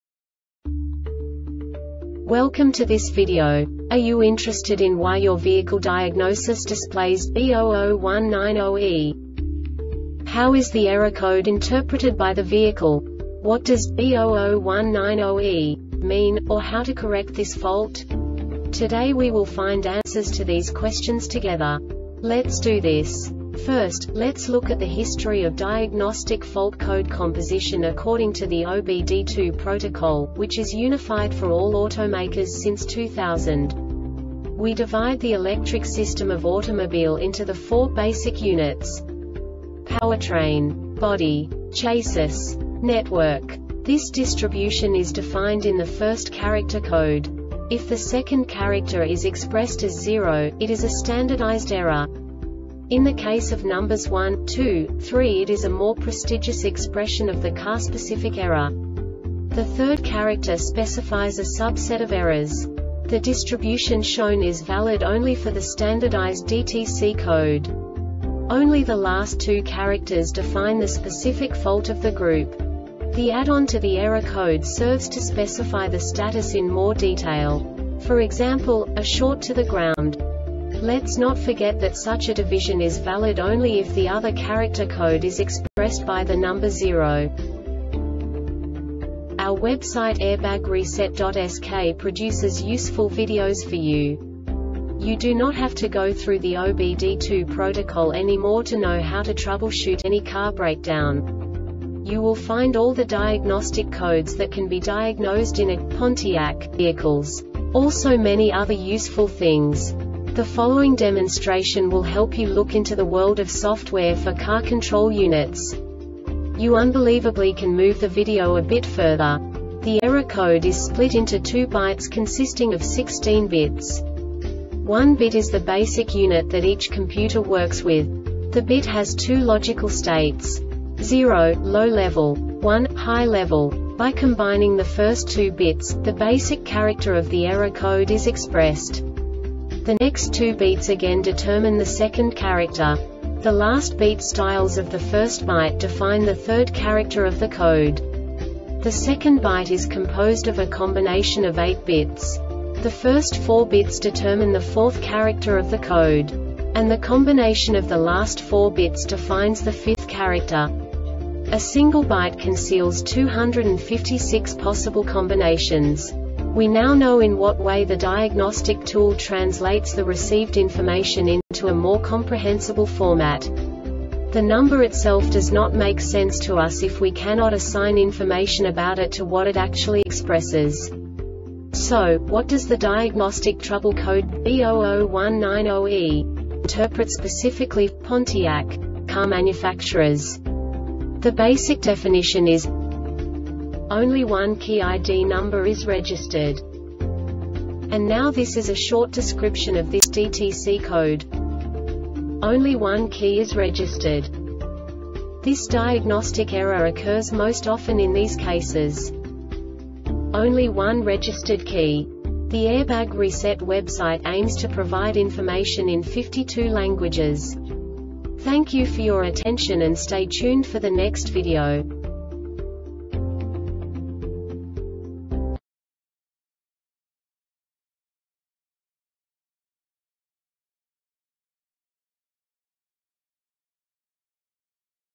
DTC Pontiac B0019-0E Short Explanation
Contents:
0:21 Basic DTC analysis according to OBD2 protocol standard.
1:48 Insight into programming
2:58 A diagnostically understandable description of the Diagnostic Trouble Code
3:05 Basic error definition

Make:
Pontiac
Code:
B0019 0E
Definition:
Only One Key ID Number Is Registered
Description:
Only one key is registered
Cause:
 Only one registered key